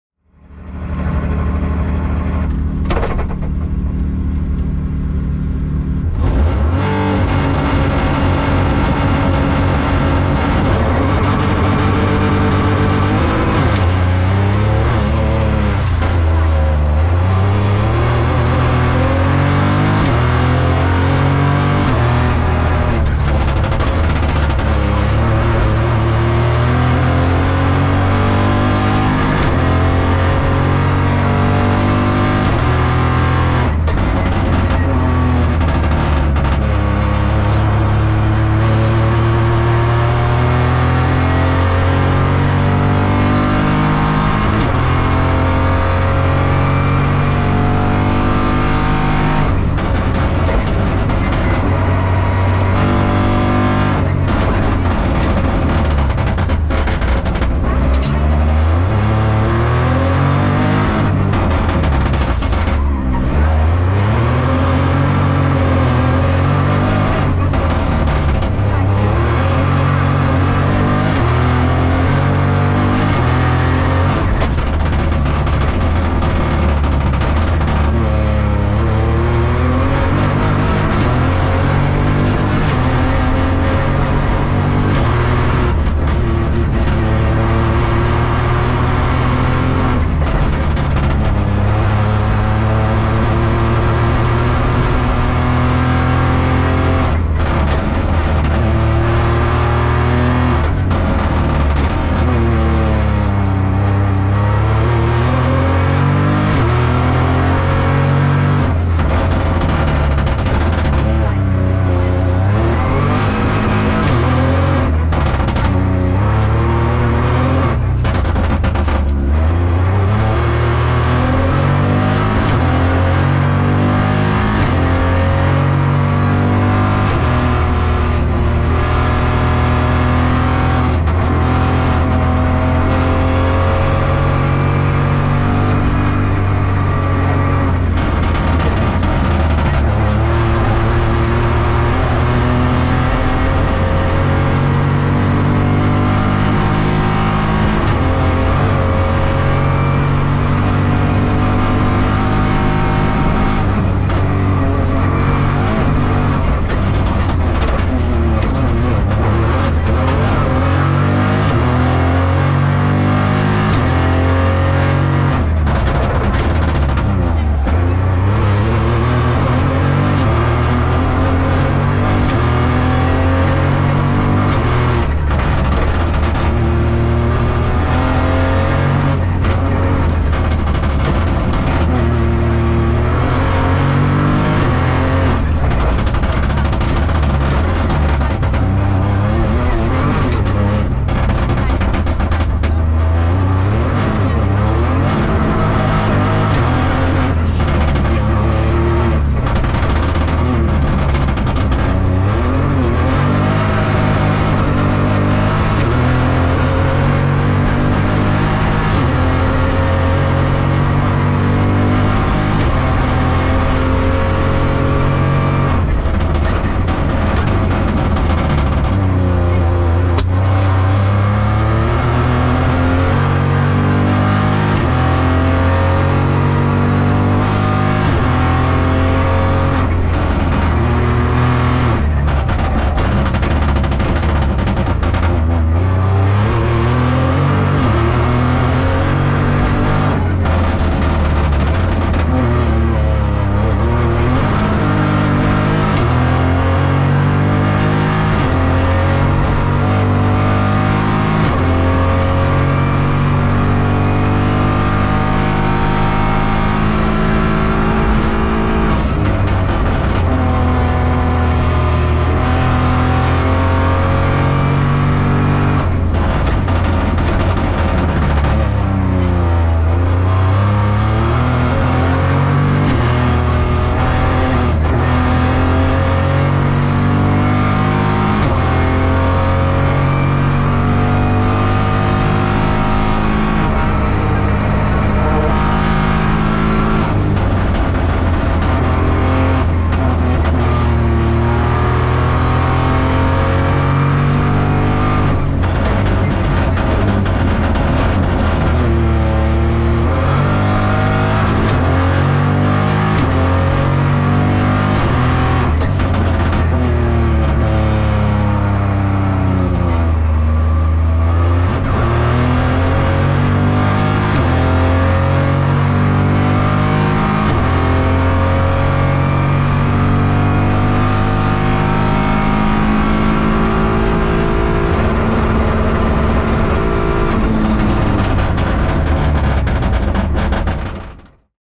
ADAC Knaus-Tabber 3 Städte Rallye | WP9 Perlesreut | Merkel & Kiefer | 3te Zeit
Gegen Ende der WP Zeit verloren um sicher zu gehen das bei einem Klassenkonkurrent alles gut ist. Sportlich und Fair!